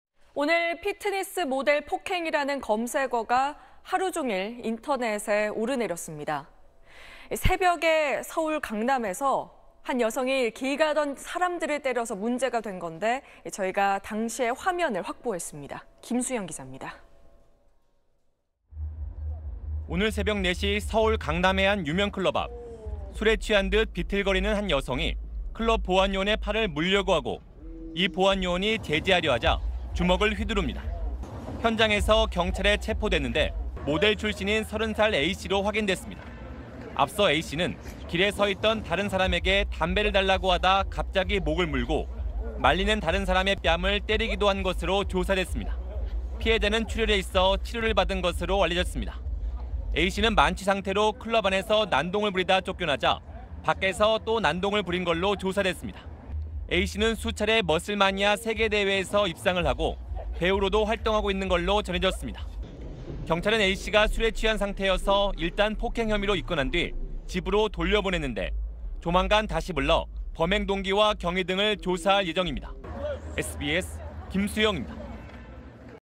행인 목 물고, 뺨 때리고…'머슬마니아' 만취 난동 당시 / SBS
오늘(22일) 피트니스 모델 폭행이라는 검색어가 하루 종일 인터넷에 오르내렸습니다. 새벽에 서울 강남에서 한 여성이 길 가던 사람들을 때려서 문제가 된 건데, 저희가 당시의 화면을 확보했습니다.
김수영 기자입니다.